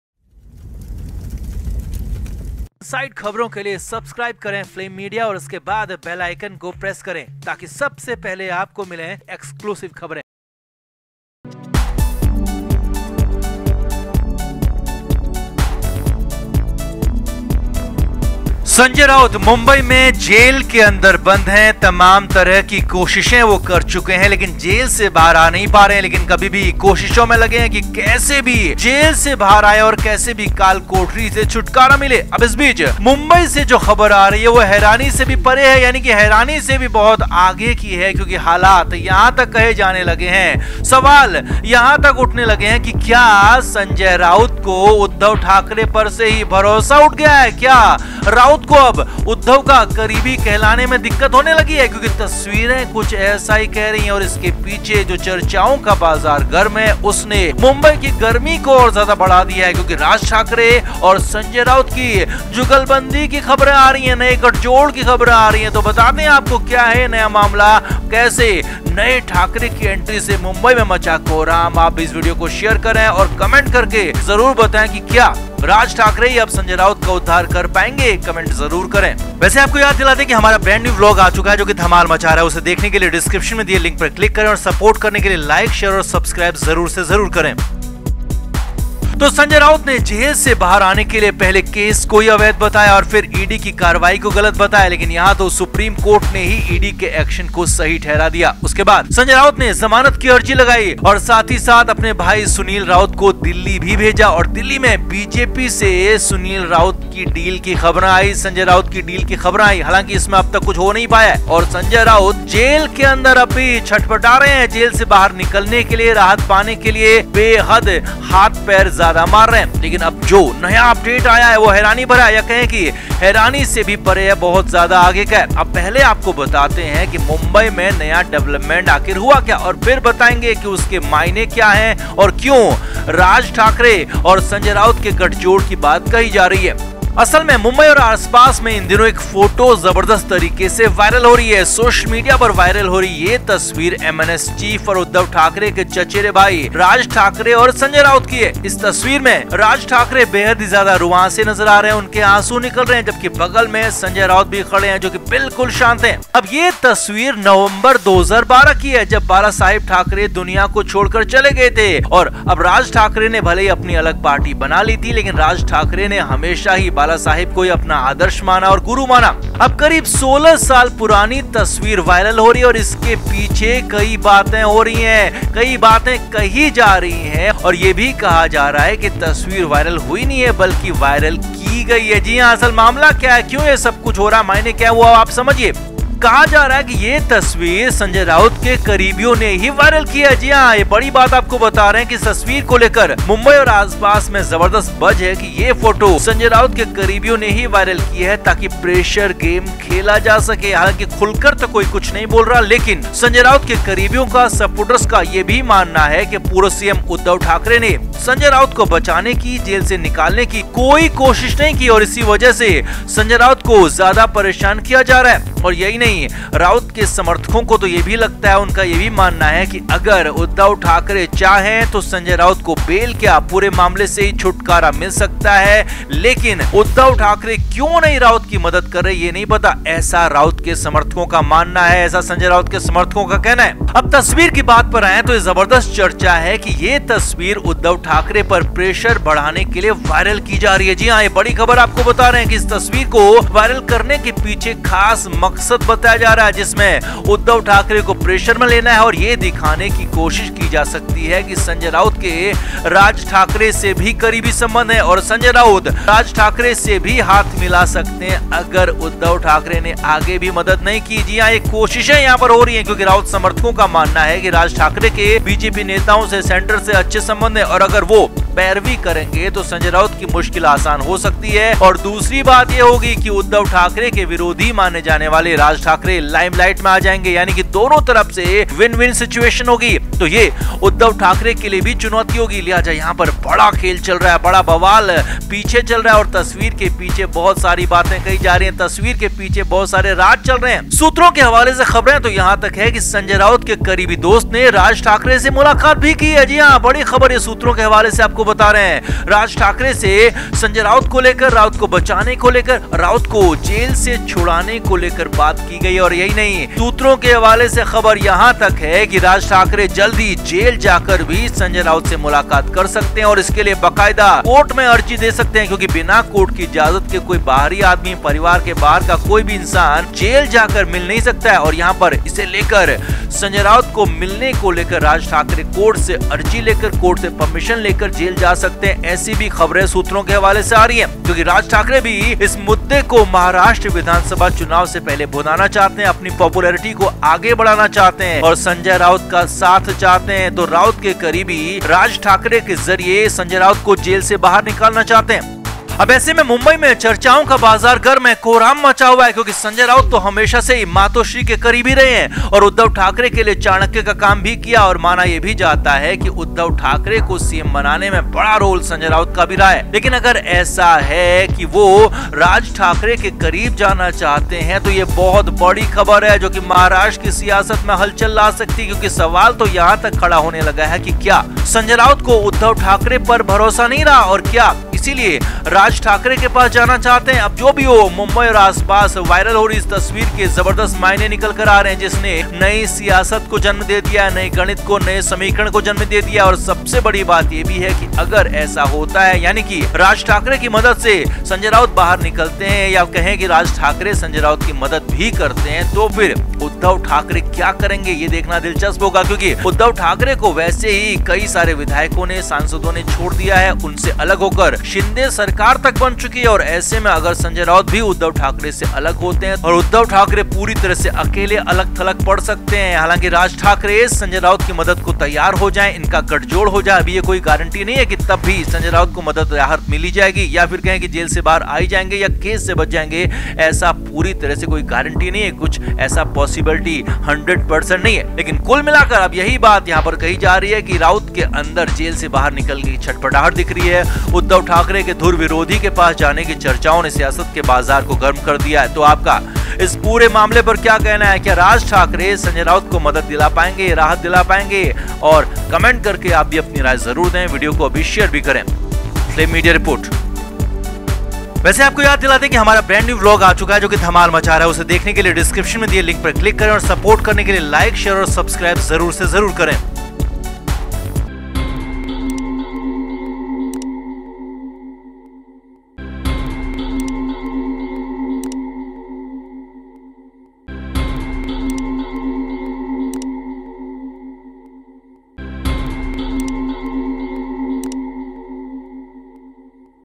उद्धव ठाकरे के बजाय अब राज के करीब जा रहे संजय राउत?.. नई चर्चाओं ने मुंबई में बढ़ाई गर्मी
हमारा नया Vlog यहां देखें- 👇🏻👇🏻👇🏻👇🏻👇🏻